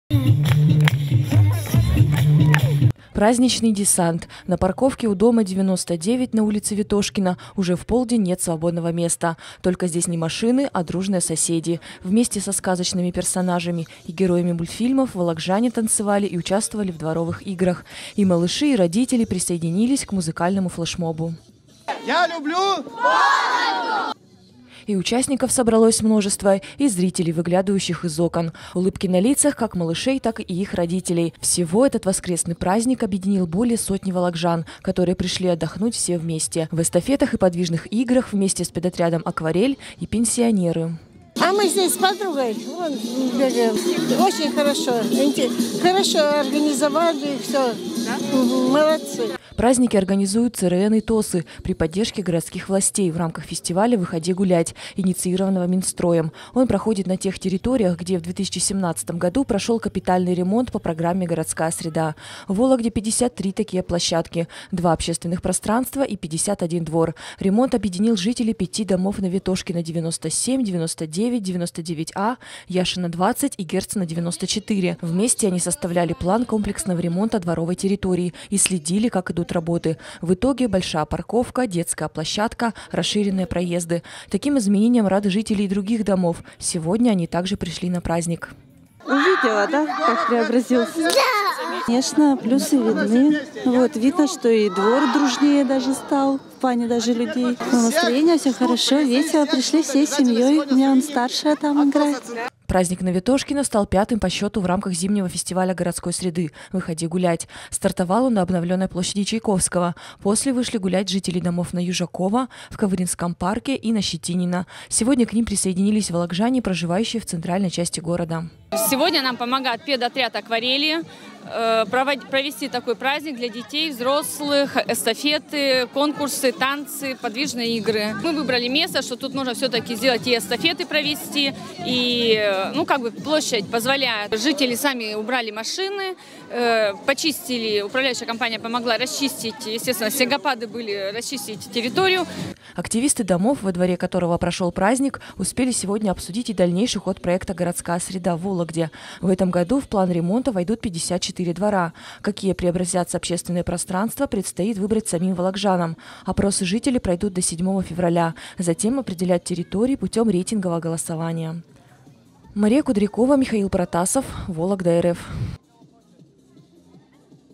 В любви своему городу признались сегодня более 100 участников фестиваля «Выходи гулять»
Семейный праздник прошел сегодня во дворе дома № 99 по ул. Ветошкина. Взрослые и дети веселились с вожатыми педотряда «Акварели». Этот праздник стал пятым по счету в рамках зимнего фестиваля городской среды «Выходи гулять».